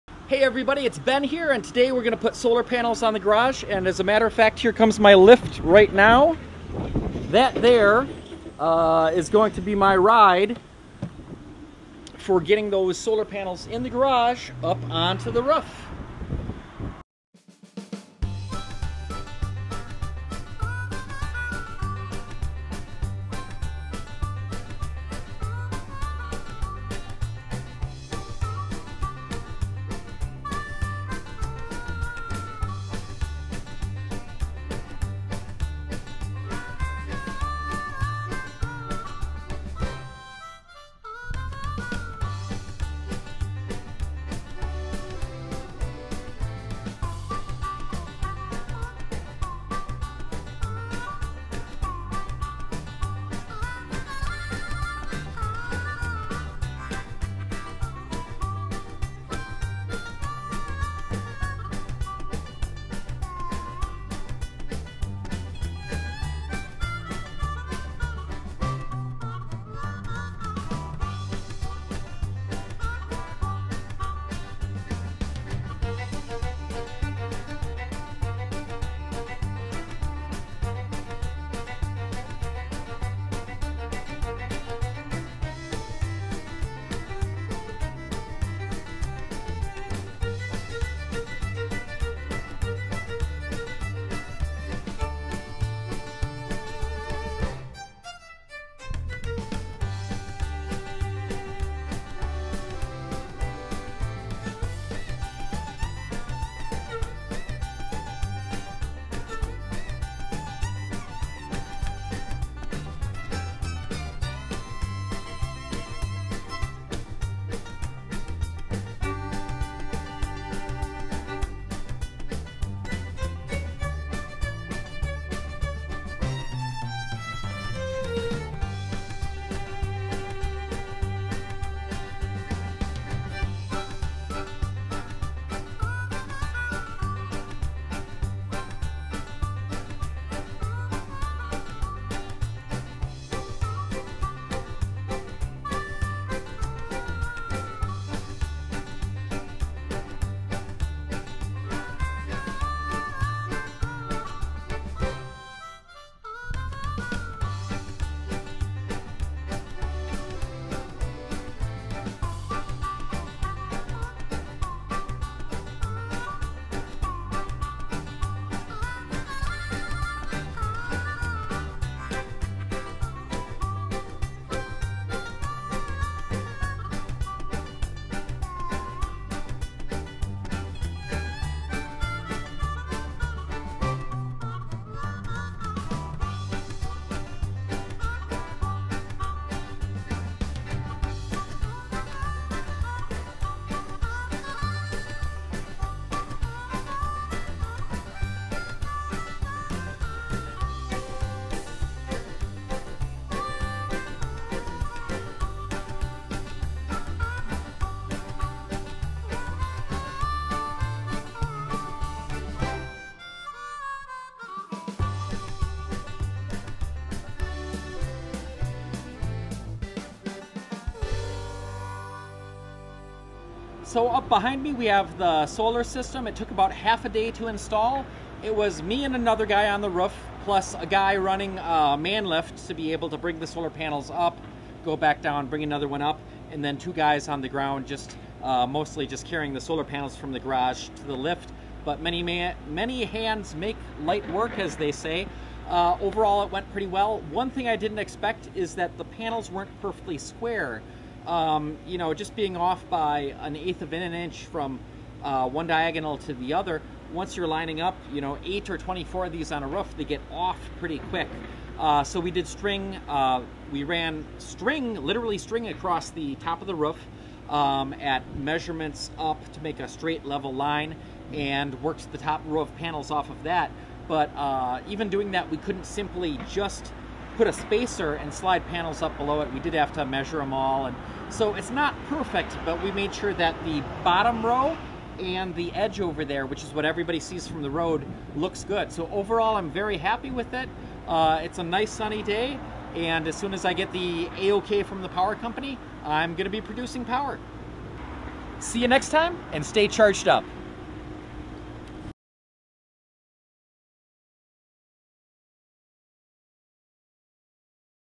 Garage Solar Panel Installation Day
We finally get to install the solar panels on the roof of the garage!
This is 24 Helios brand 260 watt 60 cell solar modules connected to Enphase M215 micro-inverters and mounted on Iron Ridge XR-100 racking.

I actually got my electric inspection and utility testing done the very next day! You can now view power production of the solar panels